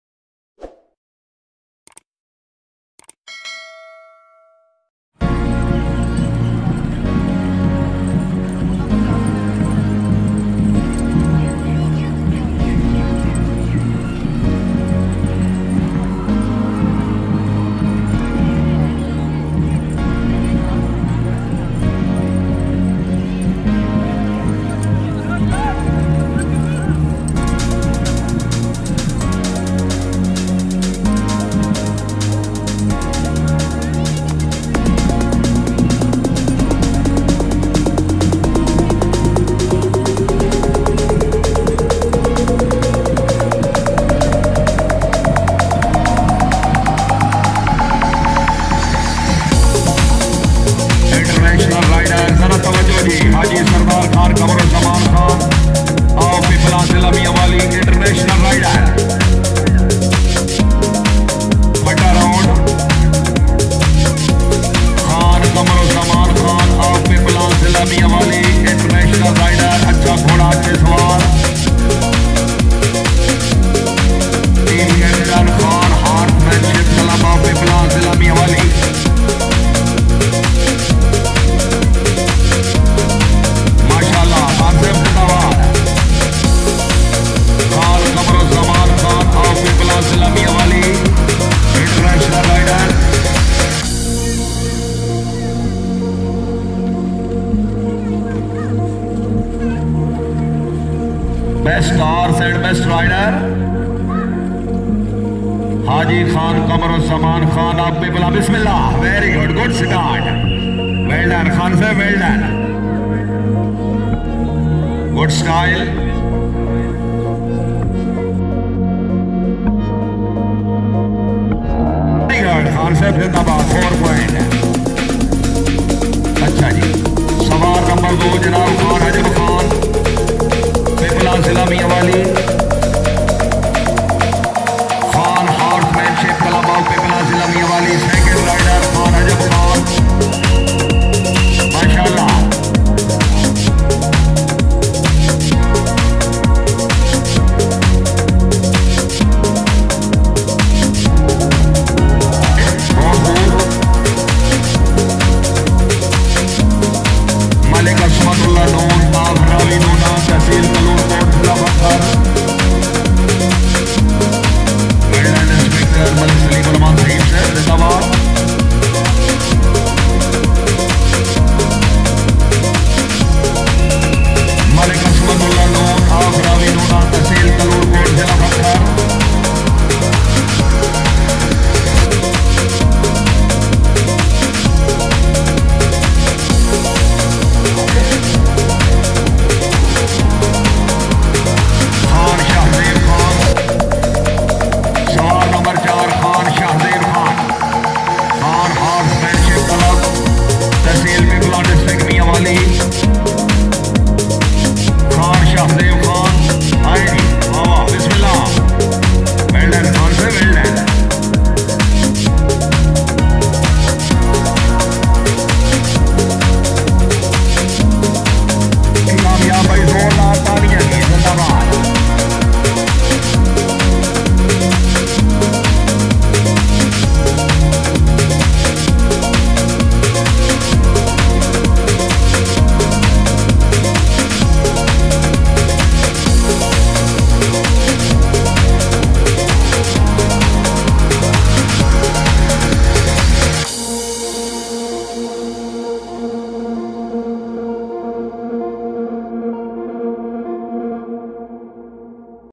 National Championship Lahore | Neza bazi | Horse Riding Skills | Pakistan Tent Pegging
khan horsemanship club
riders= Qamar Zaman Khan
 Ajab khan
Malik Asmatullah noon
Shahzaib Khan
Mela held at ranger head quarter lahore